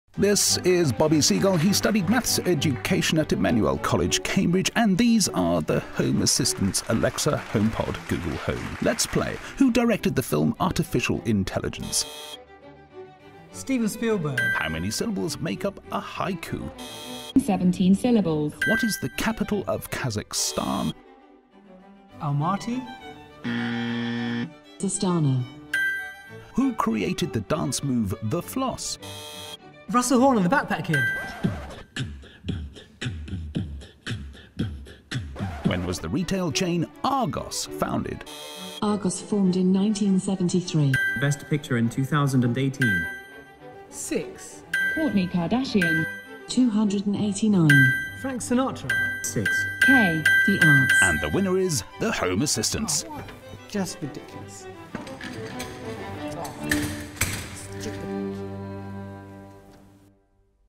Quiz show legend takes on home assistant devices - who is smarter?
Cambridge University graduate and quiz show master Bobby Seagull goes up against Amazon Alexa, Apple HomePod and Google Home in the ultimate battle of the wits. Who will be crowned the winner?